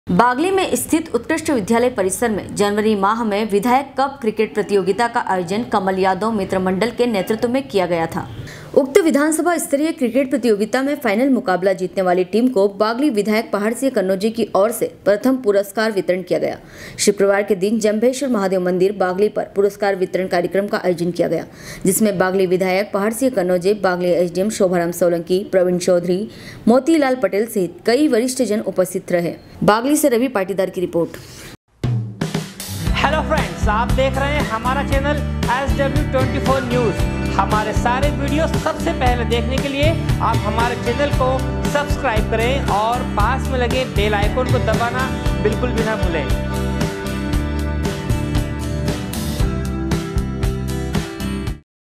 SW 24 News Madhyapradesh
बागली में स्थित उत्कृष्ट विद्यालय परिसर में जनवरी माह में विधायक कप क्रिकेट प्रतियोगिता का आयोजन कमल यादव मित्र मंडल के नेतृत्व में किया गया था। बागली से रवि पटीदार की रिर्पोट।